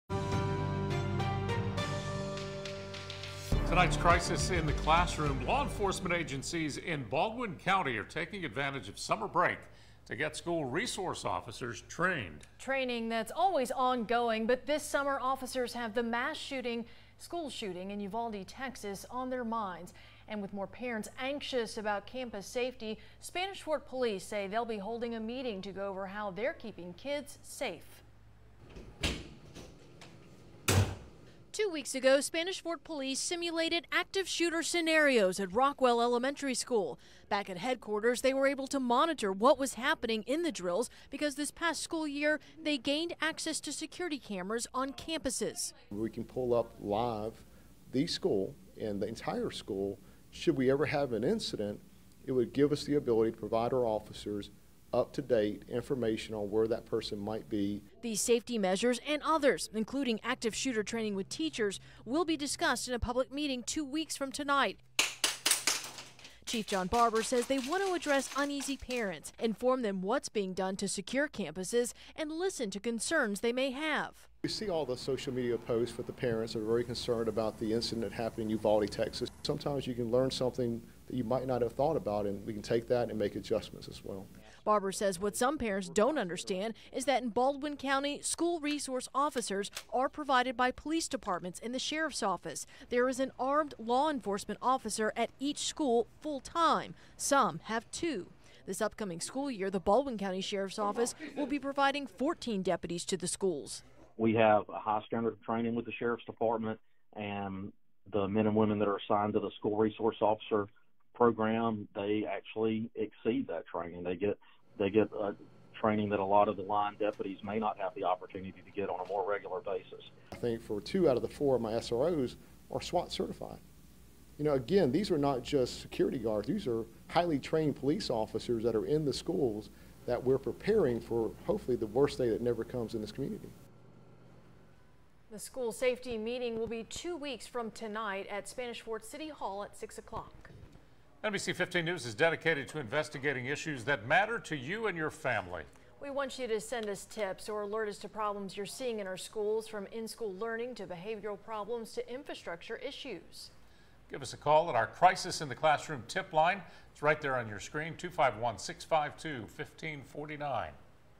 Spanish Fort Police planning school safety meeting for parents - NBC 15 WPMI
Two weeks ago Spanish Fort Police simulated active shooter scenarios at Rockwell Elementary. Back at headquarters they were able to monitor what was happening in the drills because this past school year Chief John Barber says the department gained access to security cameras on campuses.

"We can pull up live, the school and the entire school. Should we ever have an incident, it would give us the ability to provide our officers up to date information on where that person might be," said Barber.